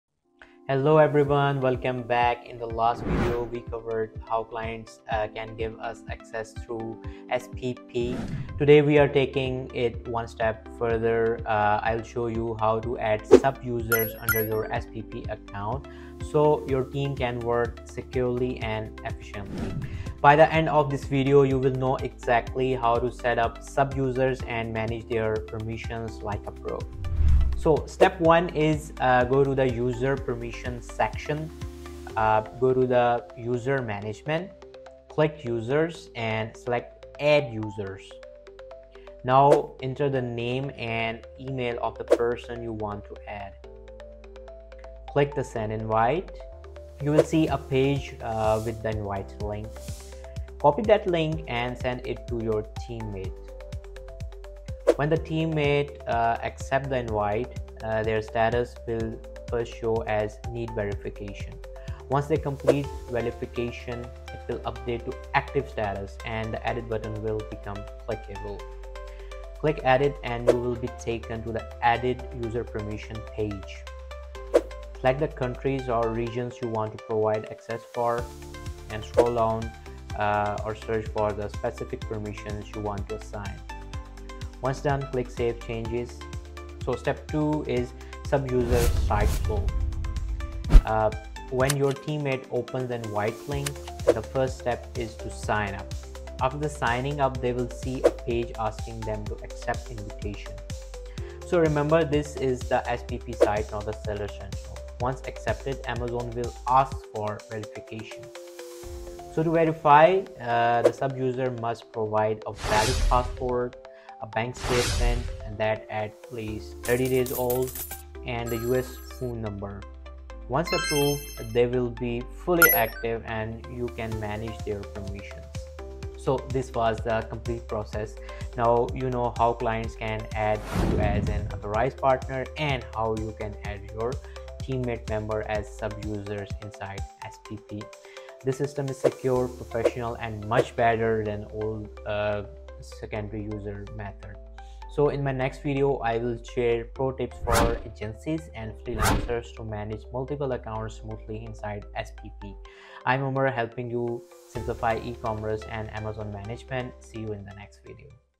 How to Add Sub-Users in Amazon SPP Account (Step-by-Step Tutorial) | Part 3
Learn how to add sub-users under your Amazon SPP (Service Provider Platform) account with this easy step-by-step guide. In this video, I’ll walk you through:

✅ Navigating User Permissions & Management
✅ Sending and accepting user invitations
✅ Setting permissions by country/region
✅ Sub-user sign-up, verification, and approval process
✅ Why SPP is more secure than the old secondary user access method

This tutorial is perfect for agencies, freelancers, and Amazon service providers who want to manage teams and client accounts more securely.

👉 Watch till the end for insights on managing multiple accounts smoothly inside SPP!